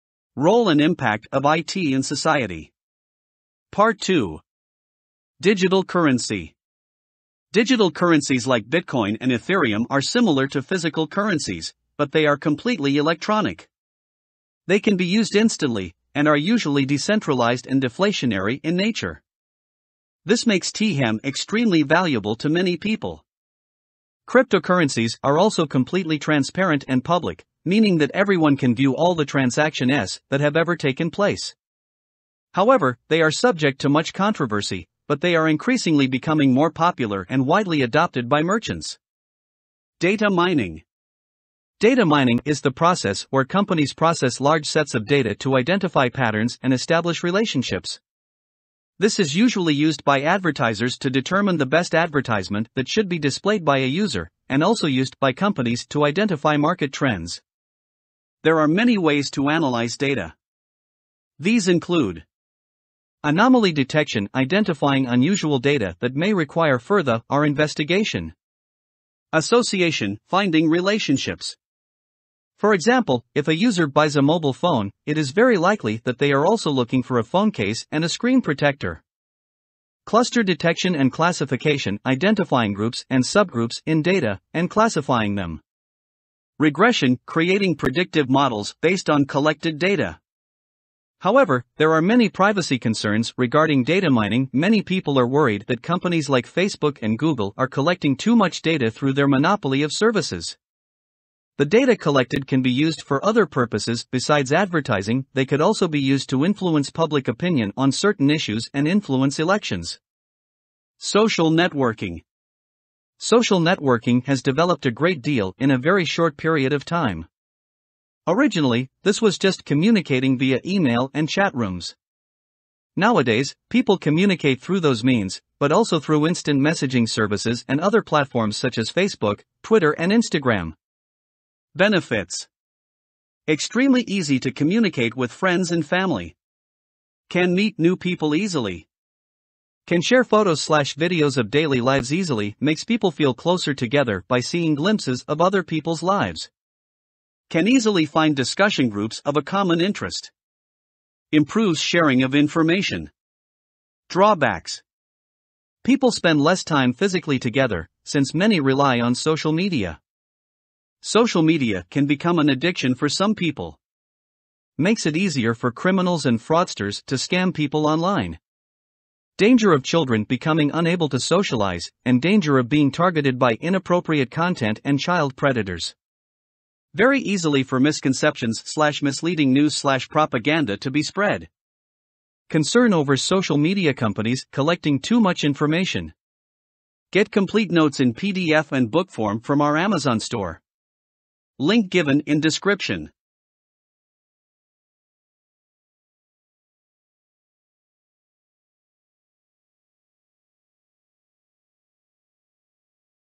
Role and Impact of IT in Society Part 2 | A Level Notes Information Technology 9626
Digital Currency

 Digital currencies like Bitcoin and Ethereum are similar to physical currencies, but they are completely electronic. They can be used instantly, and are usually decentralized and deflationary in nature. This makes t hem extremely valuable to many people. Cryptocurrencies are also completely transparent and public, meaning that everyone can view all the transaction s that have ever taken place. However, they are subject to much controversy, but they are increasingly becoming more popular and widely adopted by merchants.

Data Mining

Data mining is the process where companies process large sets of data to identify patterns and establish relationships. This is usually used by advertisers to determine the best advertisement that should be displayed by a user, and also used by companies to identify market trends. There are many ways to analyse data.

These include:

● Anomaly detection - identifying unusual data that may require furthe r investigation.

● Association - finding relationships. For example, if a user buys a mobile phone, it is very likely that they are also looking for a phone case and a screen protector.

● Cluster detection and classification - identifying groups and subgroups in data, and classifying them.

 ● Regression - creating predictive models based on collected data.

However, there are many privacy concerns regarding data mining - many people are worried that companies like Facebook and Google are collecting too much data through their monopoly of services. The data collected can be used for other purposes besides advertising - they could also be used to influence public opinion on certain issues and influence elections.

Social Networking

 Social networking has developed a great deal in a very short period of time. Originally, this was just communicating via email and chat rooms. Nowadays, people communicate through those means, but also through instant messaging services and other platforms such as Facebook, Twitter and Instagram.

 Benefits

● Extremely easy to communicate with friends and family.

● Can meet new people easily.

● Can share photos/videos of daily lives easily - makes people feel closer together by seeing glimpses of other people’s lives.

 ● Can easily find discussion groups of a common interest.

● Improves sharing of information.

 Drawbacks

● People spend less time physically together, since many rely on social media.

● Social media can become an addiction for some people.

● Makes it easier for criminals and fraudsters to scam people online.

● Danger of children becoming unable to socialize, and danger of being targeted by inappropriate content and child predators.

 ● Very easily for misconceptions/misleading news/propaganda to be spread.

 ● Concern over social media companies collecting too much information.